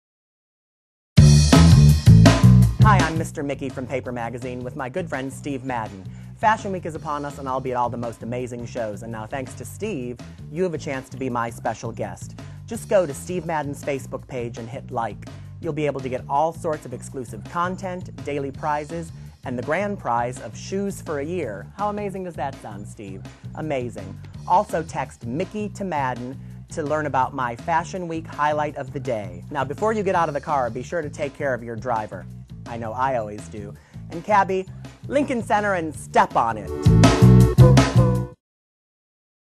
Steve Madden & PAPERMAG's Ad in NYC TaxiTV!
This is the commercial that is running in taxis all over NYC! Did you happen to catch it?!